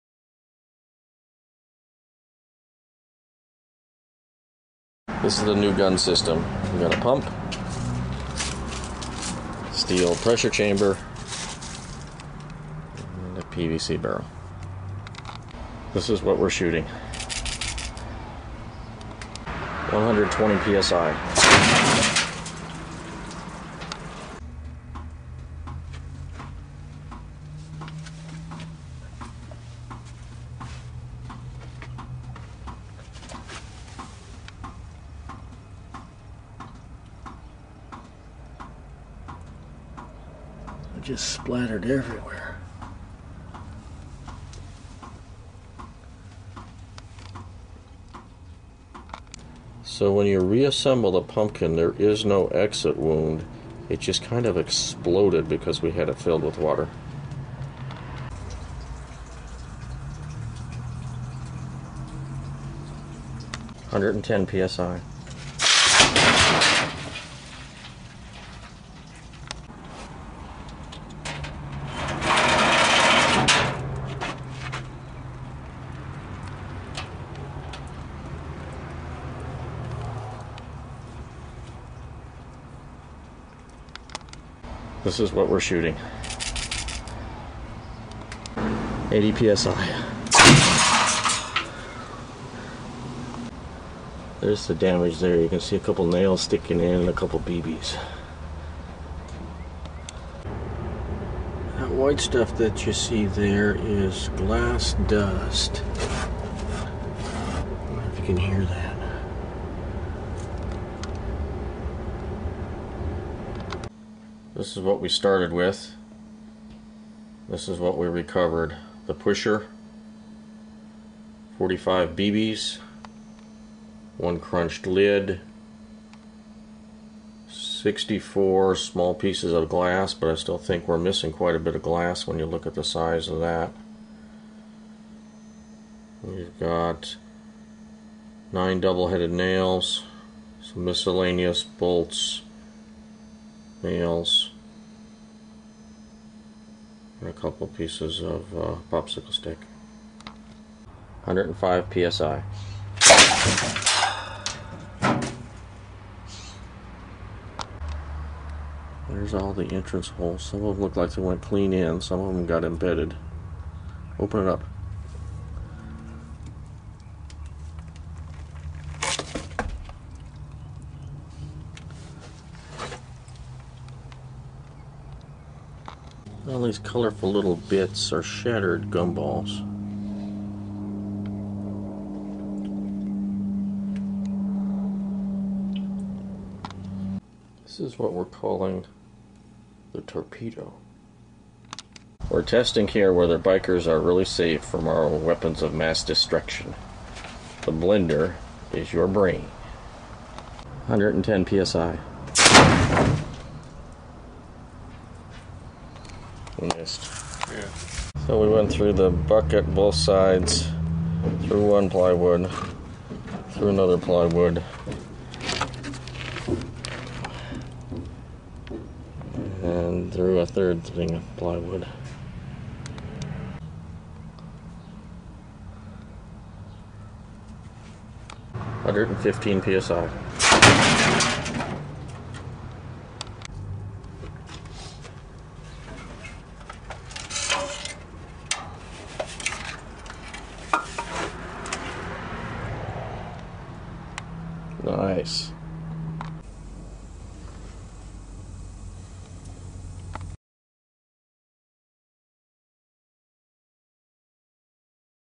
Pumpkin Carving
First videos of our new steel gun, much more power and better shots.  More power still to come!